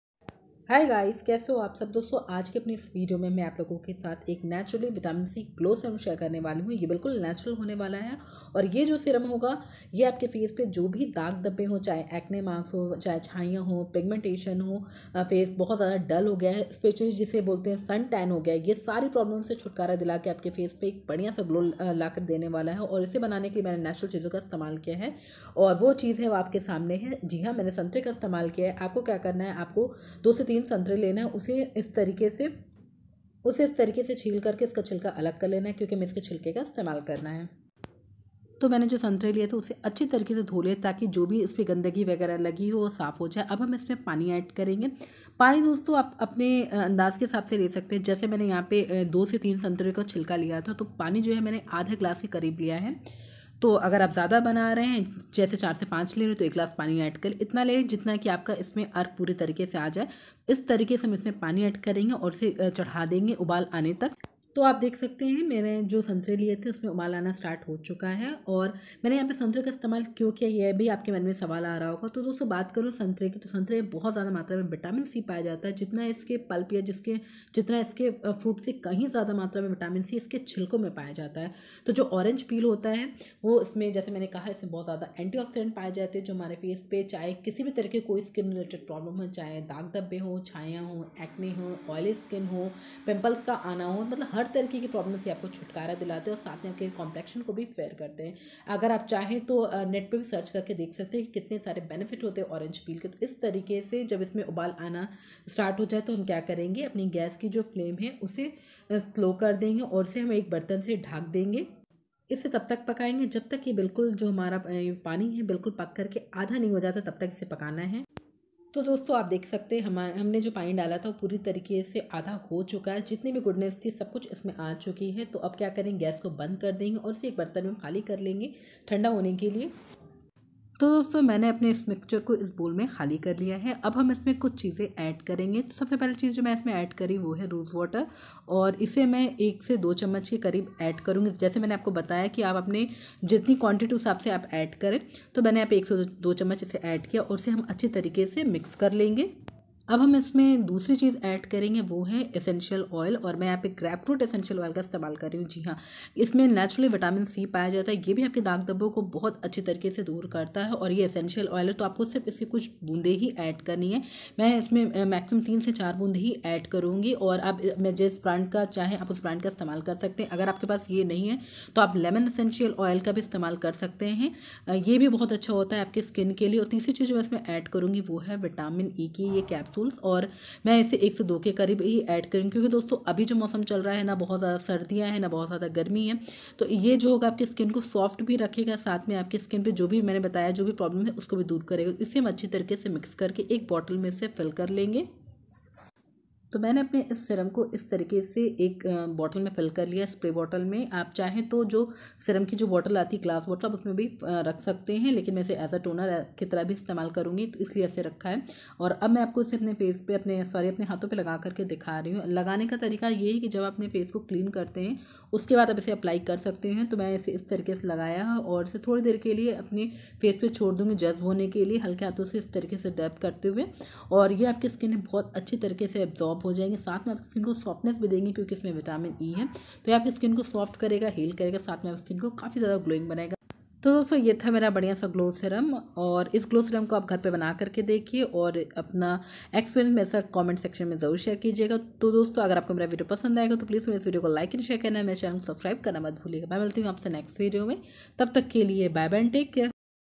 साफ चमकती त्वचा पाने के लिए घर पर बनाए ये Naturally Vitamin C Glow serum||facial serum.
About this video.👇
Dosto aj ke video me ap logo ke sath ek skin whitening glow serum ki recipe share krungi.
Thanks.